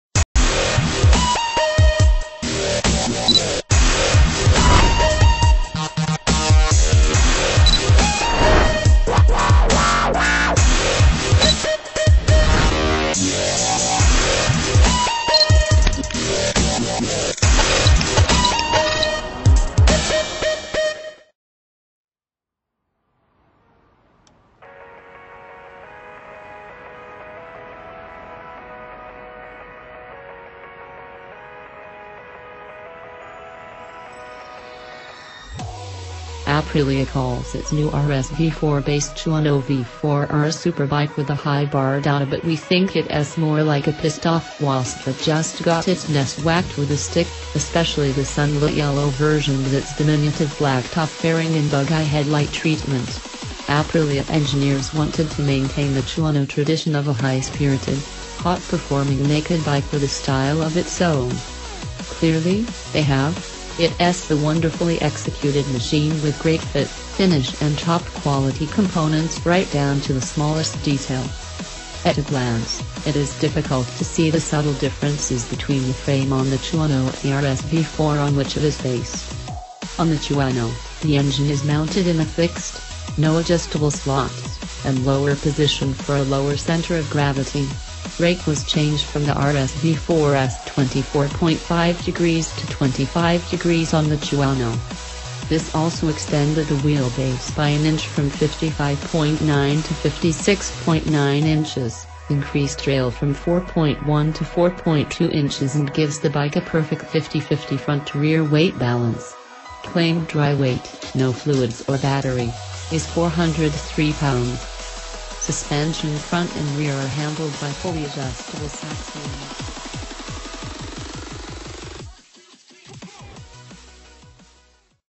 HOT INFO!!!The Best of Aprilia Tuono V4 R price and Colours Announced
HOT INFO!!!The Best of Aprilia Tuono V4 R price and Colours Announced - Aprilia calls its new RSV4-based Tuono V4 R a “superbike with a high bar.” But we think it’s more like a pissed-off wasp that just got its nest whacked with a stick, especially the sunlit-yellow version with its diminutive black-top fairing and bug-eye headlight treatment. Aprilia engineers wanted to maintain the Tuono tradition of a high-spirited, hot-performing naked bike with a style of its own. Clearly, they have; it’s a wonderfully executed machine with great fit, finish and top-quality components right down to the smallest detail.

At a glance, it is difficult to see the subtle differences between the frame on the Tuono and the RSV4 on which it is based. On the Tuono, the engine is mounted in a fixed (no adjustable slots) and lower position for a lower center of gravity. Rake was changed from the RSV4’s 24.5 degrees to 25 degrees on the Tuono. This also extended the wheelbase by an inch from 55.9 to 56.9 inches, increased trail from 4.1 to 4.2 in. and gives the bike a perfect 50/50 front-to-rear weight balance. Claimed dry weight (no fluids or battery) is 403 pounds.

Rs. 2225473